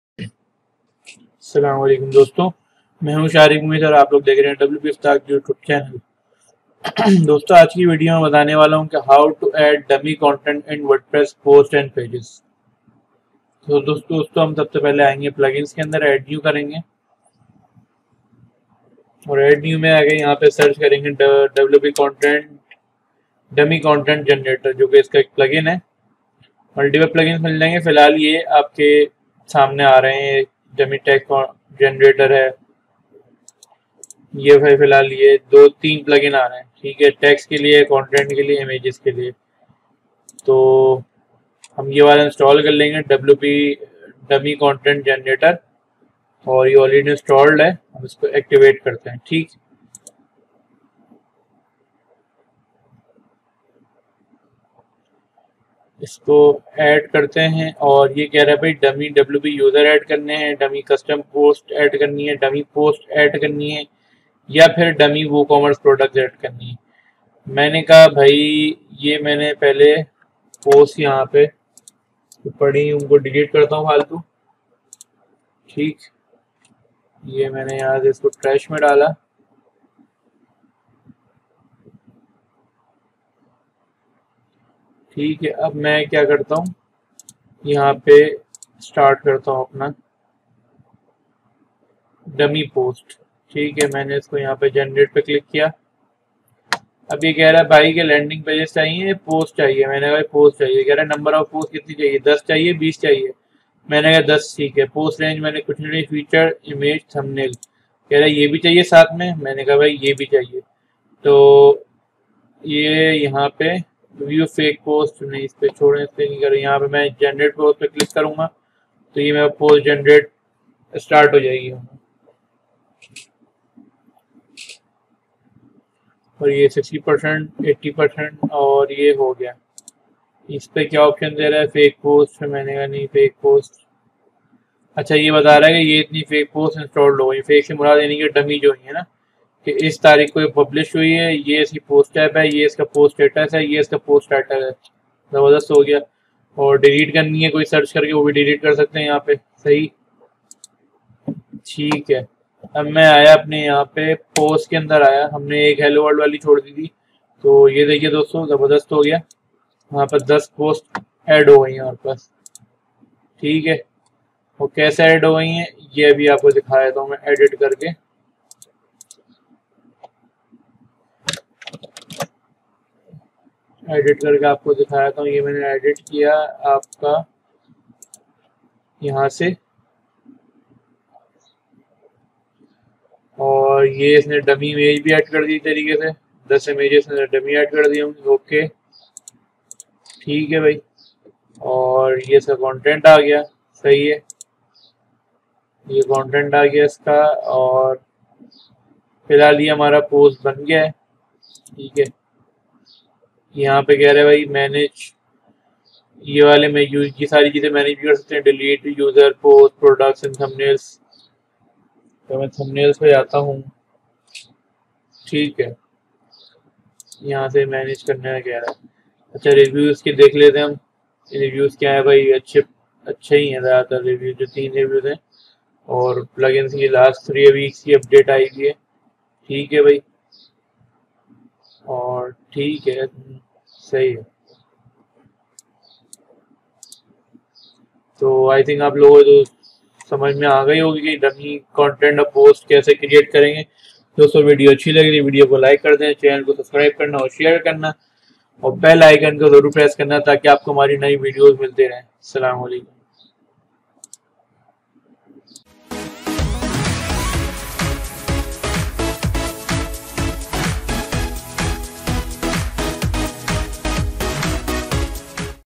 How To Add Dummy Content In Wordpress | Dummy Images Creator | In Urdu/Hindi 2022
In this  video you will learn how to add dummy content in wordpress & dummy images in wordpress in your website by using free plugins.

Wordpress in Urdu /  Hindi Tutorials  by WP USTAAD